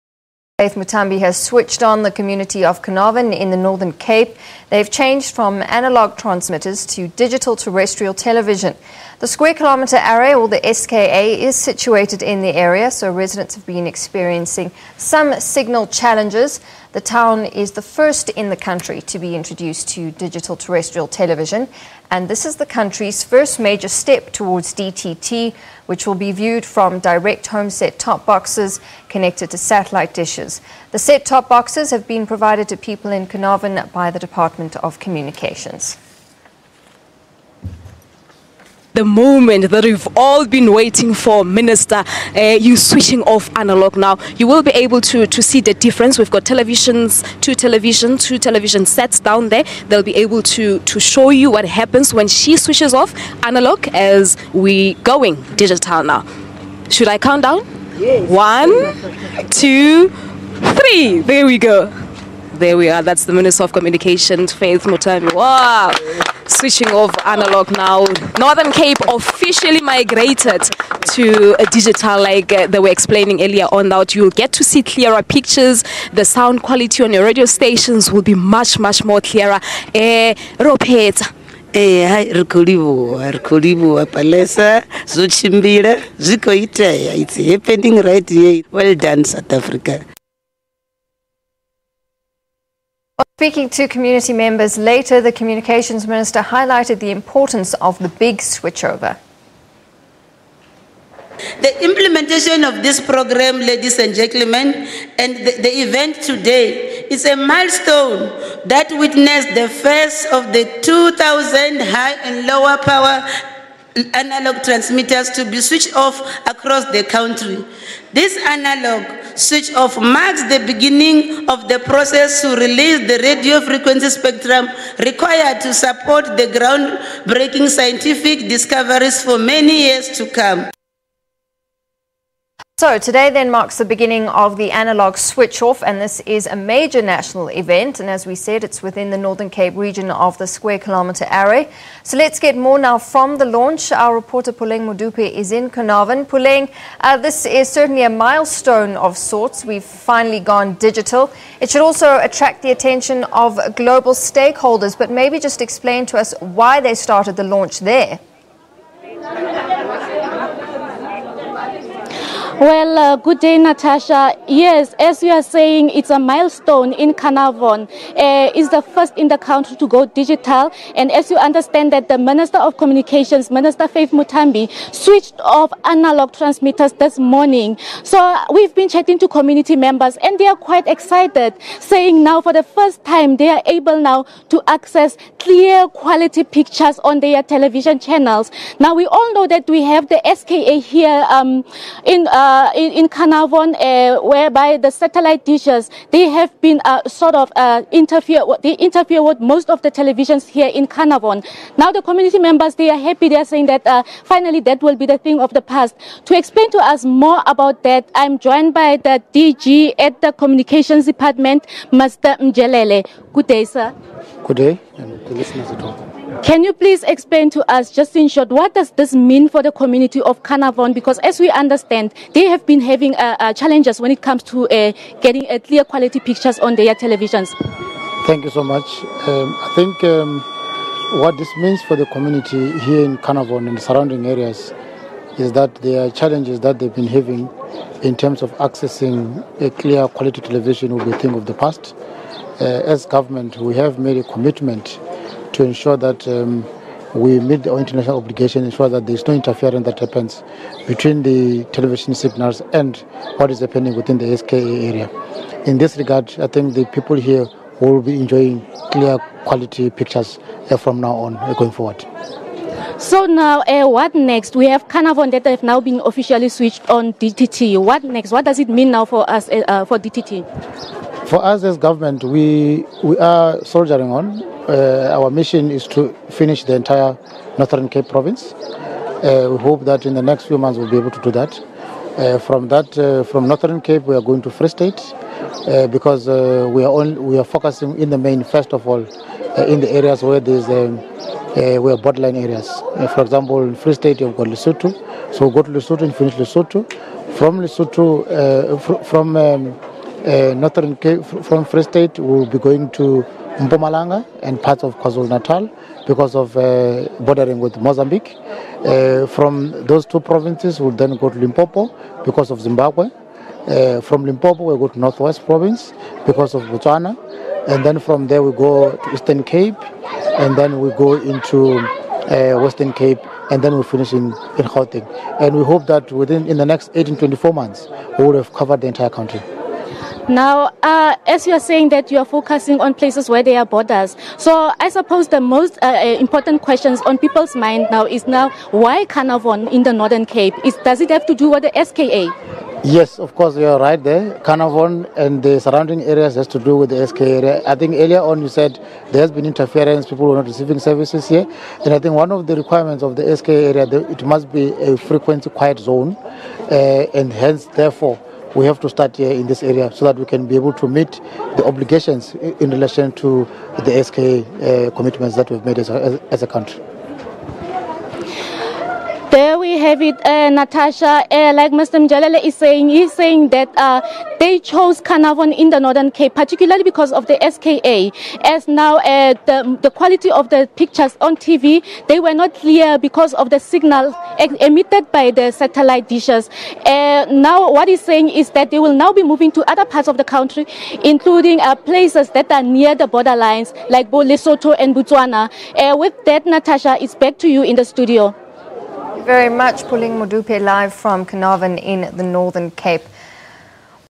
SA goes digital....DTT launch in Carnarvon
Communications Minister, Faith Muthambi, has switched-on the community of Carnarvon in the Northern Cape.
They've changed from analogue transmitters to Digital Terrestrial Television.